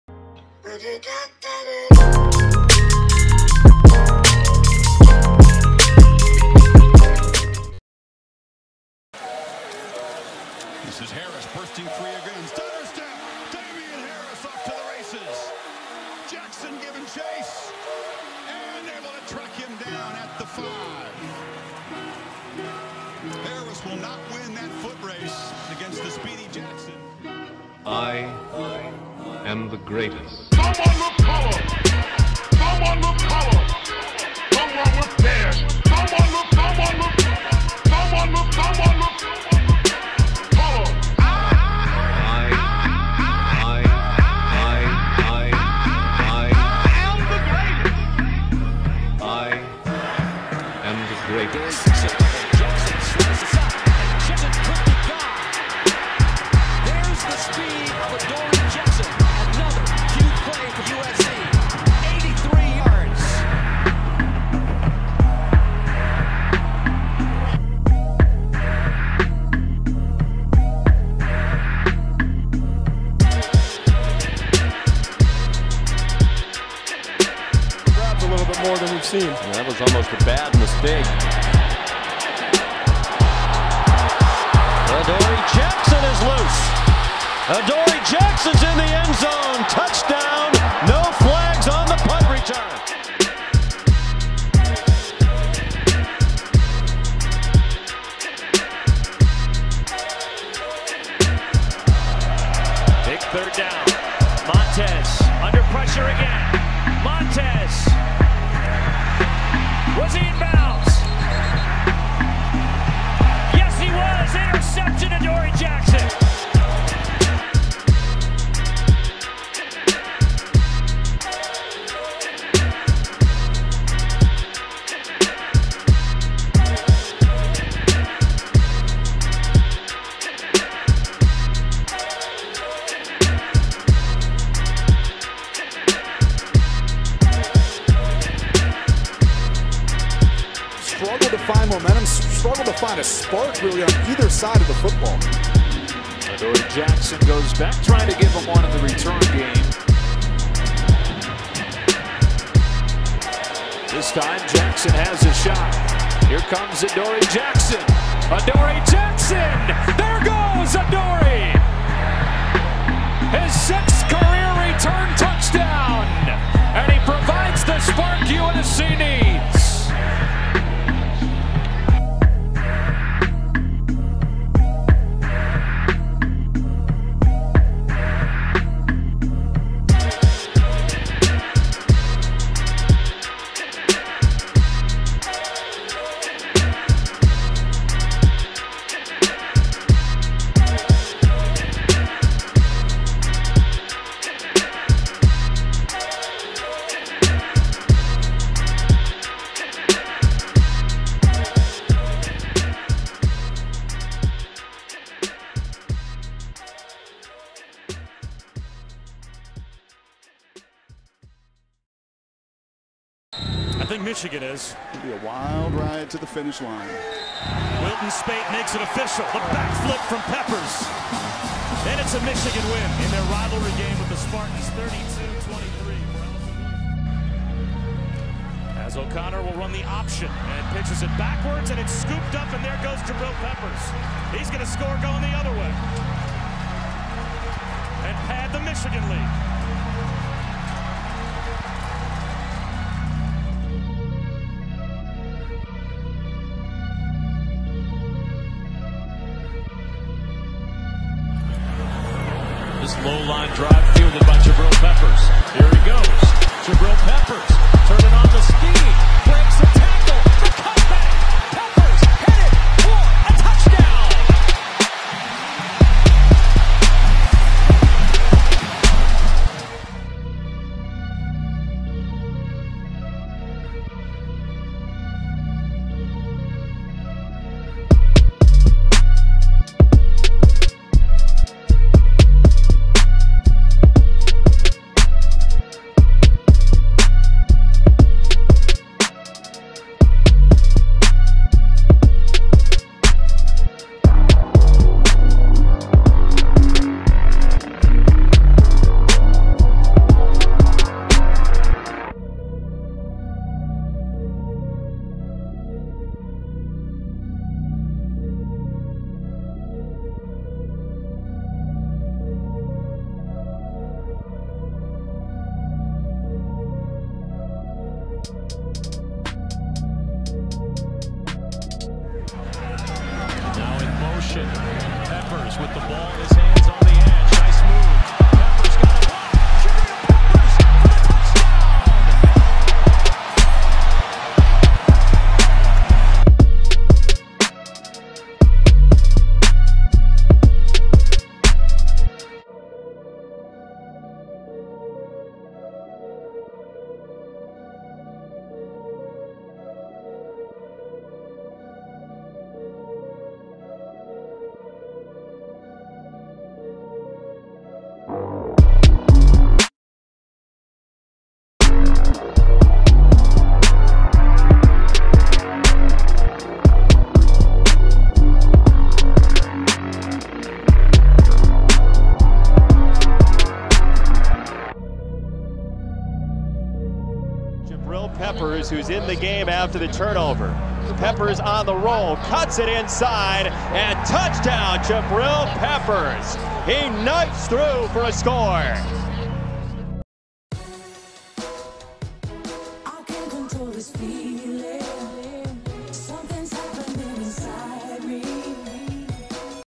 Jabrill Peppers vs. Adoree' Jackson
Jabrill Peppers has stormed onto the college football scene as a threat at more than 5 positions, playing offense, defense, and special teams. Adoree' Jackson, however, also playing many positions, has been much quieter, and has statistically put up better numbers. The question is. WHO IS BETTER?